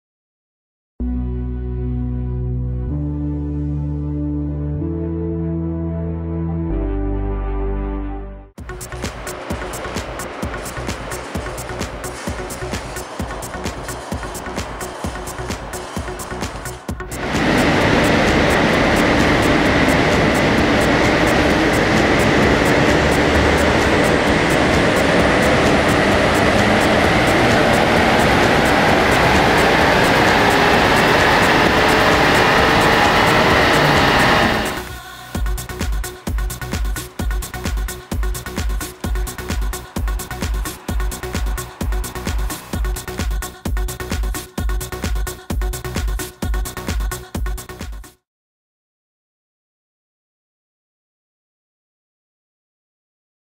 Audi A5 2.0 TDI 177cv Reprogrammation Moteur @ 219cv Digiservices Paris 77 Dyno
Passage dans nos locaux d'une Audi A5 2.0 TDI de 177cv pour une reprogrammation moteur.